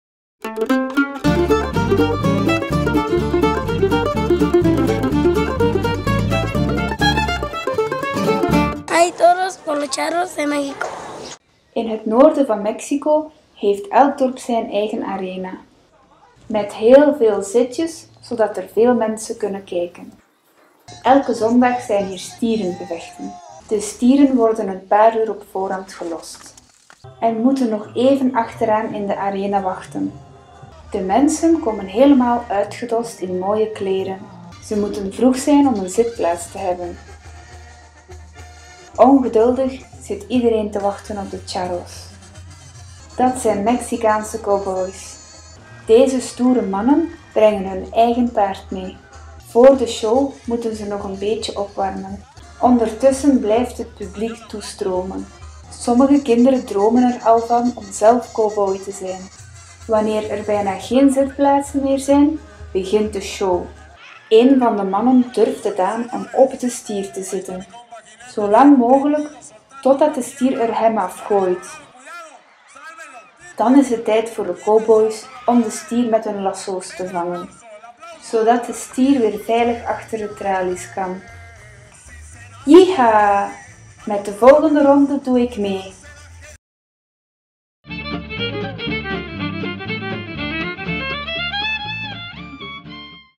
Hebben ze daar ook rodeostieren?
"Hebben ze daar ook rodeostieren?" vroeg Korneel (11j).
Kijk snel voor het antwoord!
Groetjes uit Mexico - Pablo de Kever